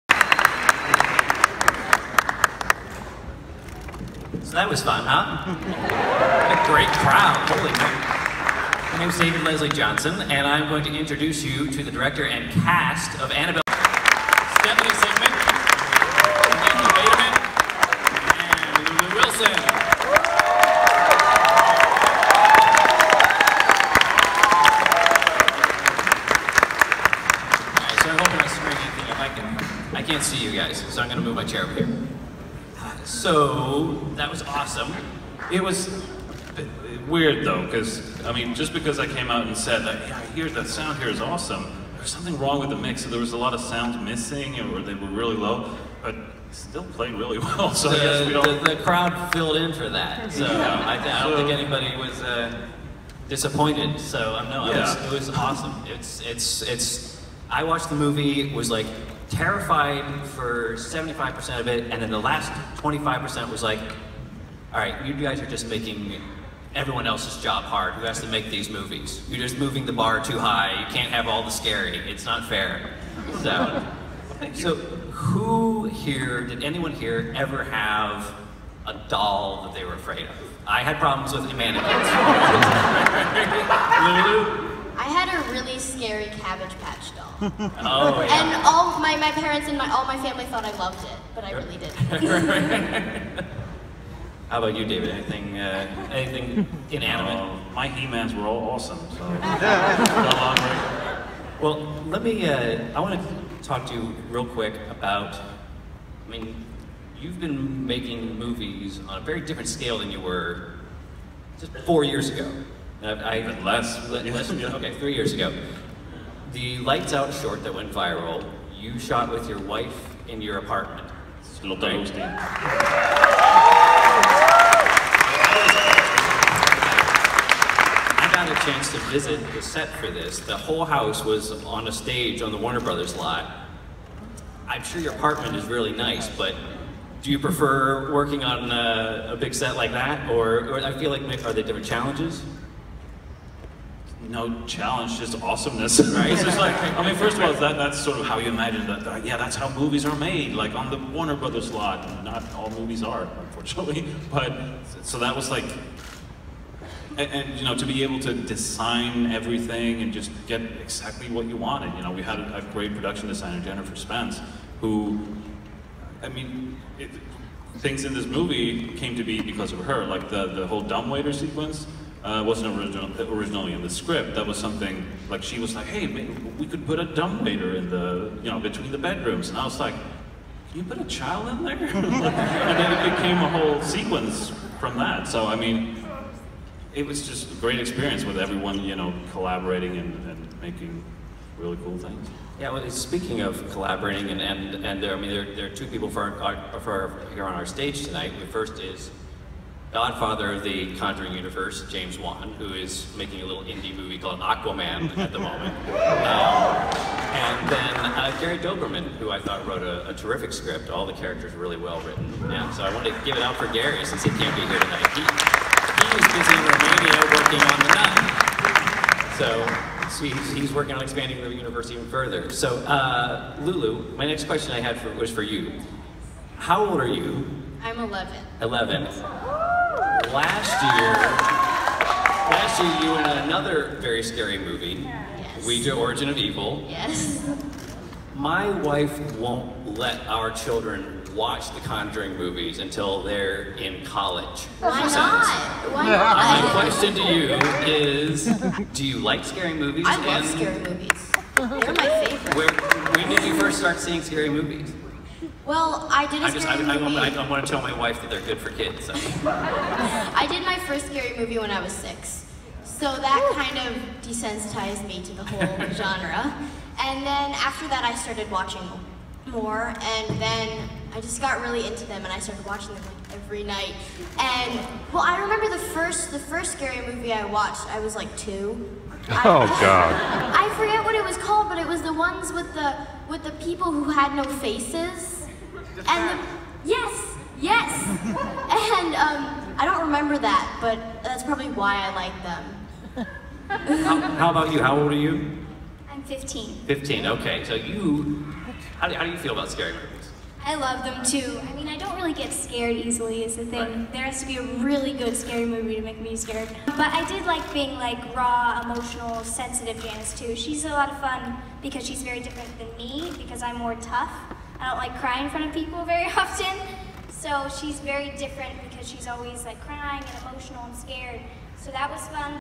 "Annabelle: Creation" Q&A, 6/19/17.
An early preview of "Annabelle: Creation" was shown on Monday evening, June 19, 2017 at The Theatre at The Ace Hotel in Downtown Los Angeles as part of the Los Angeles Film Festival.  Following the movie was a Q&A with the director, David Sandberg and the cast.  From left to right:  Moderator David Leslie Johnson, Sandberg,  Miranda Otto (Mrs. Mullins), Anthony LaPagilia (Samuel Mullins), Stephanie Sigman (Sister Charlotte), 15-year-old Talitha Bateman (Janice) and 11-year-old Lulu Wilson (Linda).  Clip is a little over five minutes long.  "Annabelle: Creation" will open in the U.S. Friday, August 11, 2017.  Spoilers deleted.  Lulu reveals the first horror film she saw was "The Descent" (2005).